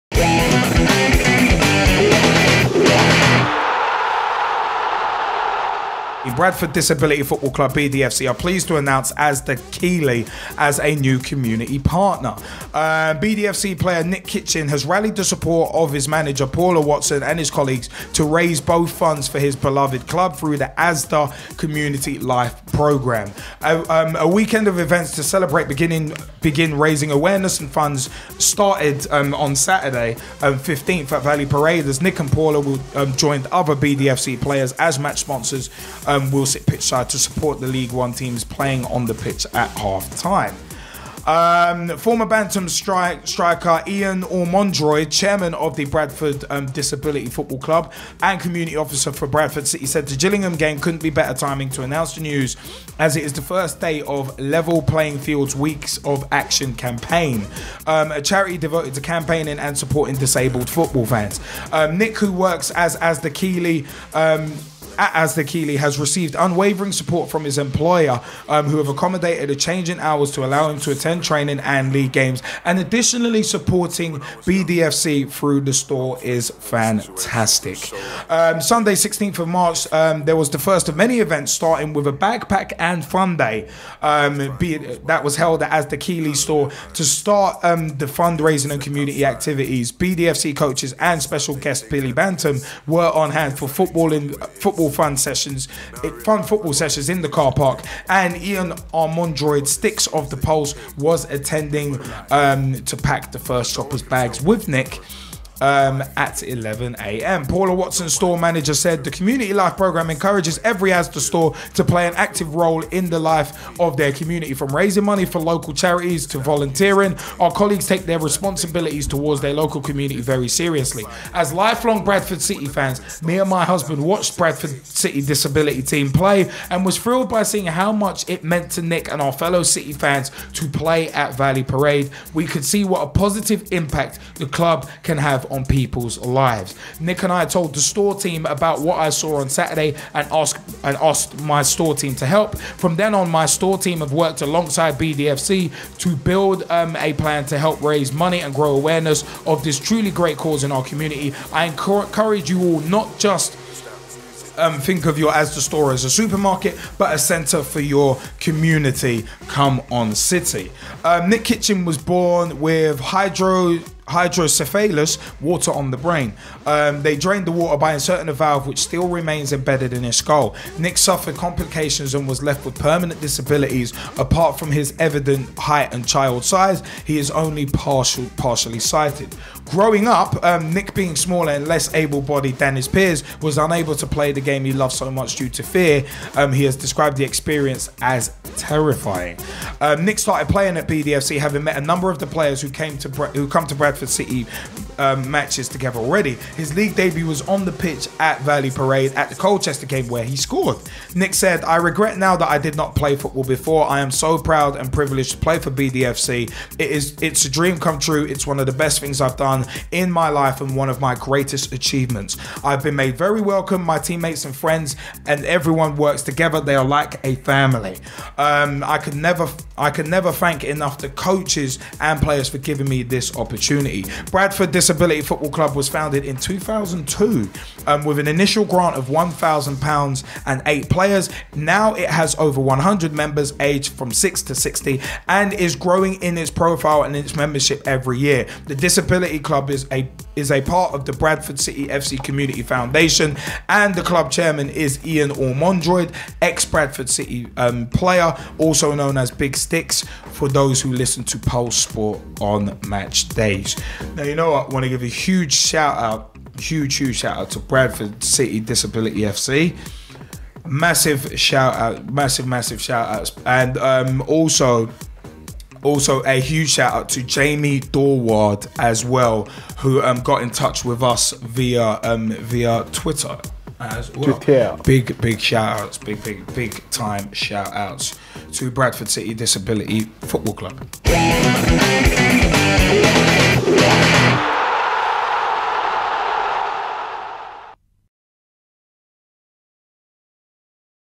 Pitch Talk 17-03-2014 - Bradford City Disability FC info
We at Pitch Talk on our 17th March 2014 show gave some information about Bradford City Disability FC & discussed ASDA Keighley becoming their newest community partner.

For info on BDFC visit -

Join us LIVE every Monday night on Youtube LIVE
(keep an eye on the links below & our Google+ page for show listen links & info)